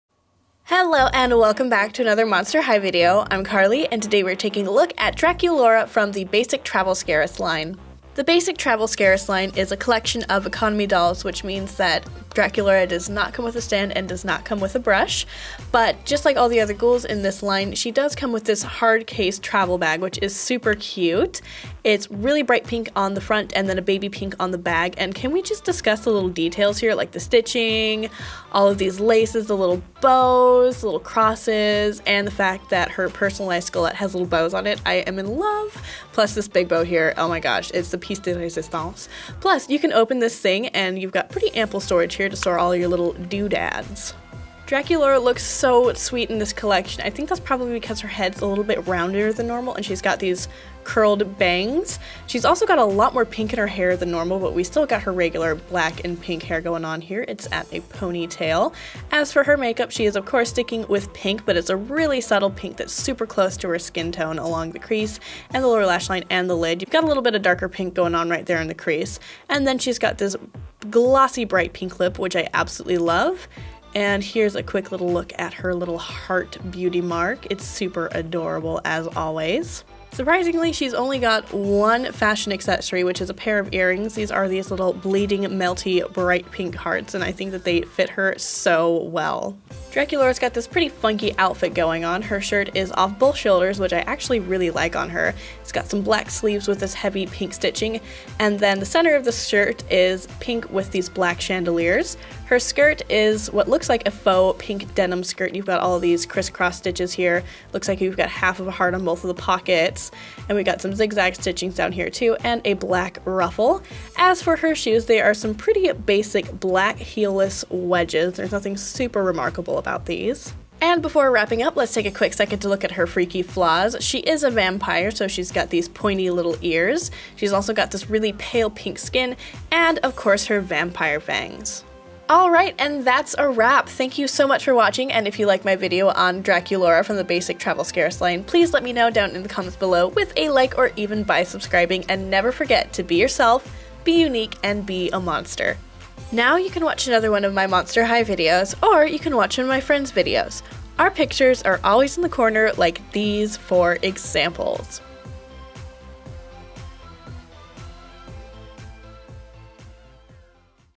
Draculaura Doll from the Monster High Basic Travel Collection!
The ghouls of Monster High are hitting the skies for their first trip abroad together in monster style. Their destination is Scaris, the city of lights and hometown for Rochelle Goyle. Draculaura, Abbey Bominable, Ghoulia Yelps and Deuce Gorgon are ready to see some scary sights dressed in new jet-setting outfits and carrying small travel bags. Dolls cannot stand alone.

Credits:
Filmed at Creative Videos, Corvallis, Oregon
Doll Presenter: Carly
Video Editor: Spencer Barrett

Another haul video from Carly who will unbox and review dolls just like other Monster High reviewers like  MommyandGracieShow, Radiojh, and  KittiesMama